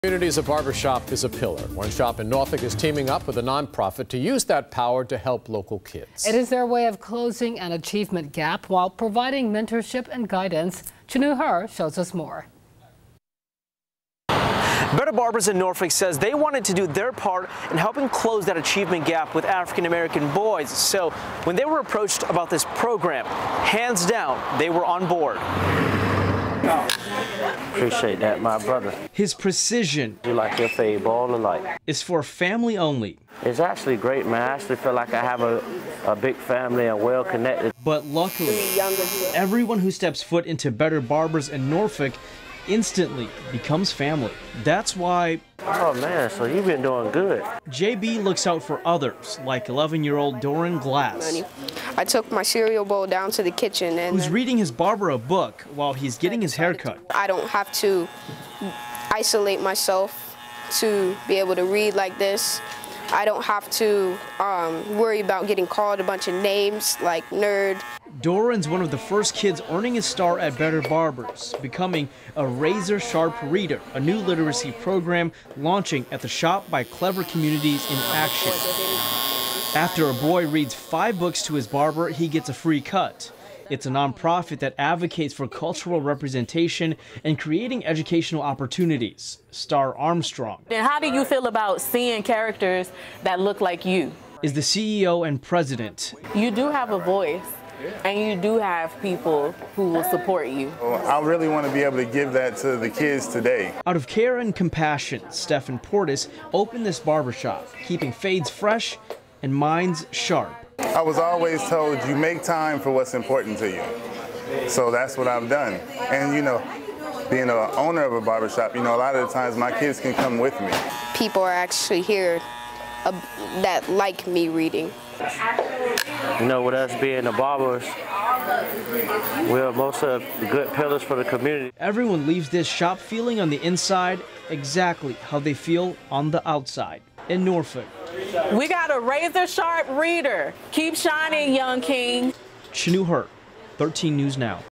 'Razor Sharp Reader' program at barber shop
It's their way of closing an achievement gap, while providing mentorship and guidance.